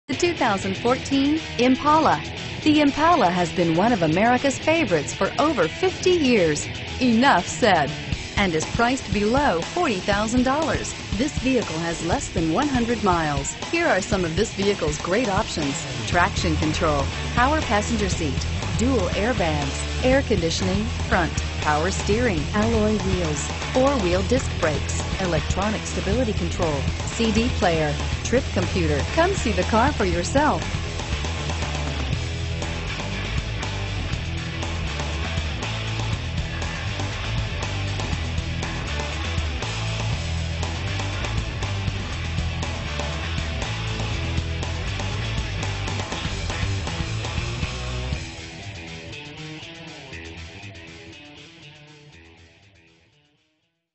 2014 Chevrolet Impala FH Dailey Chevrolet - Bay Area - San Leandro CA 5688
2014 Chevrolet Impala  - Stock#: 5688 - VIN#: 2G1155S39E9258789

Looking for the right car? Today could be your lucky day. With 3 miles, this , 2014 Chevrolet Impala equipped with Automatic transmission could be yours. Request more information and set up a test drive right away.